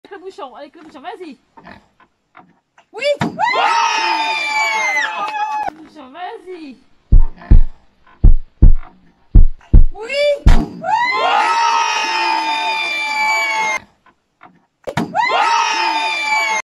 Défi watter bottle flip en Bretagne, kune kune
Eva 6 ans réussit le challenge de la bouteille d eau, admirée en partie par raiponce notre cochon.